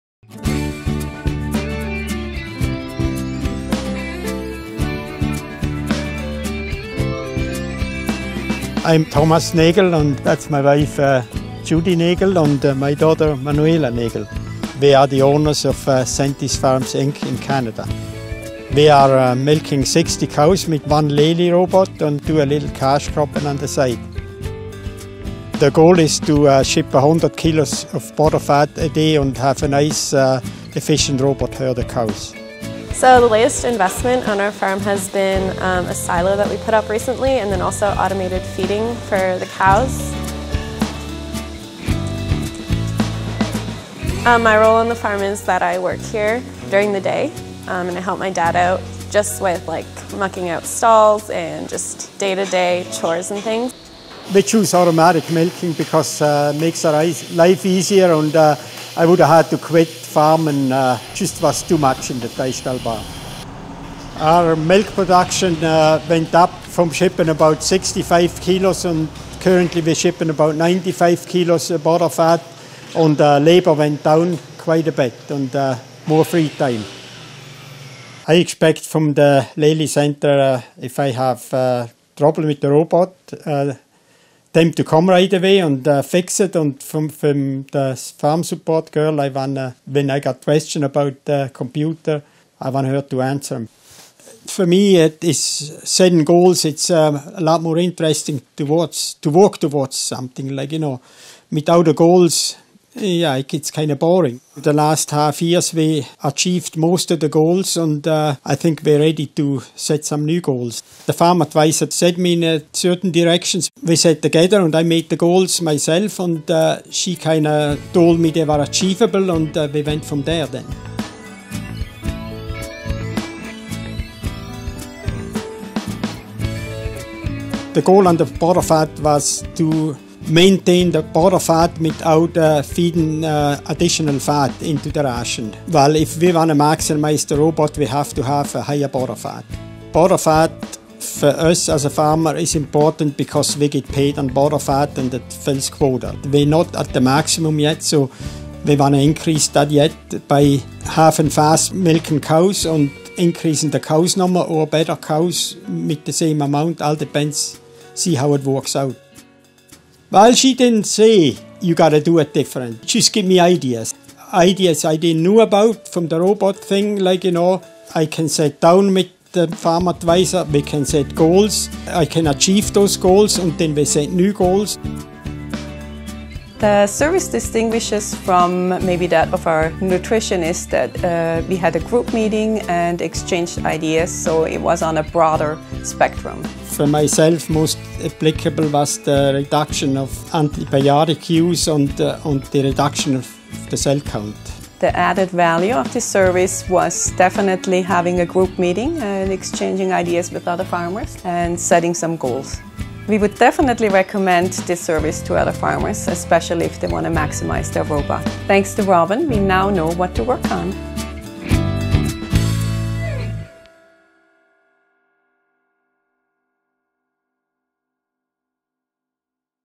Lely Farm Management Support - Testimonial Family Nagel - EN
“We now know what to focus on. I can set realistic goals and achieve them.” Thomas Nagel, Canada